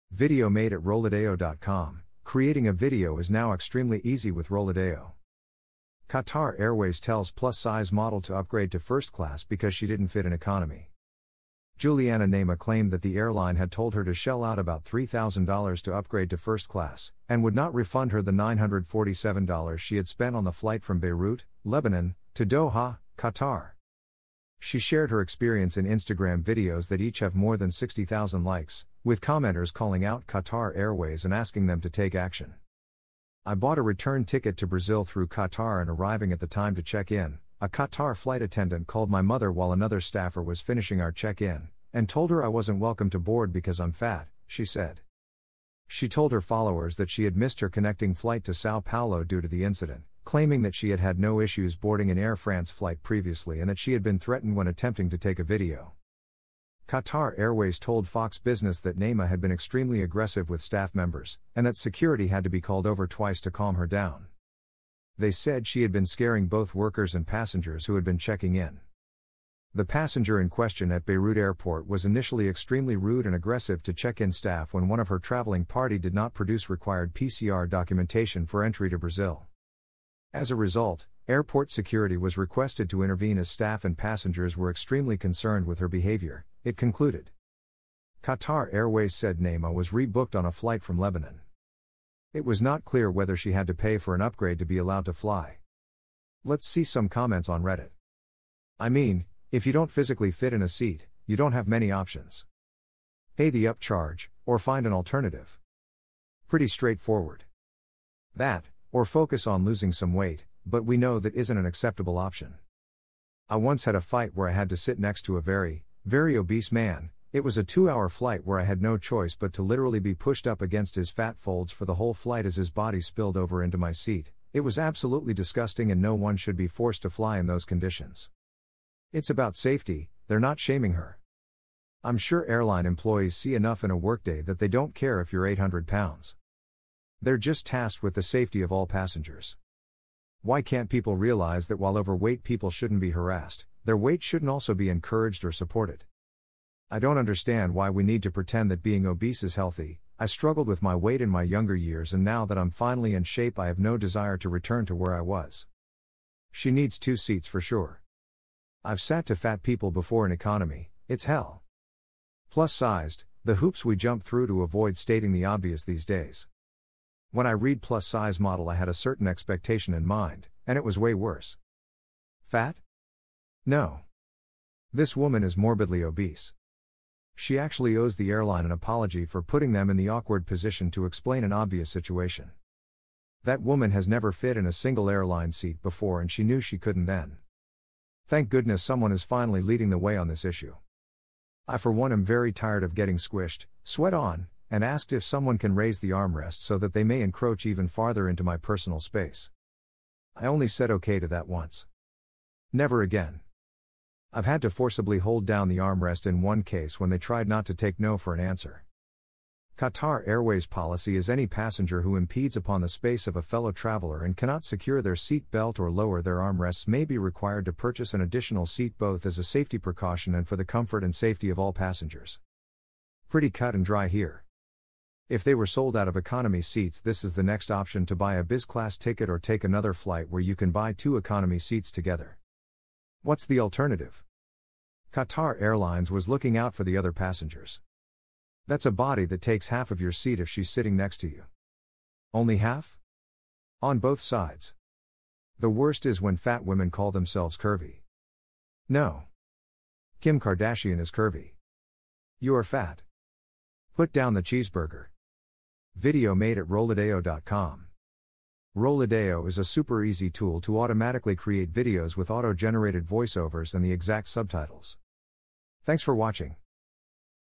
Influencer barred from flight for being 'too fat'
Qatar Airways tells plus-size model to upgrade to first class because she didn't fit in economy.

Juliana Nehme claimed that the airline had told her to shell out about $3,000 to upgrade to first class, and would not refund her the $947 she had spent on the flight from Beirut, Lebanon, to Doha, Qatar.

Video made at rollideo.com.

Rollideo is a super easy tool to automatically create videos with auto-generated voice-overs and the exact subtitles.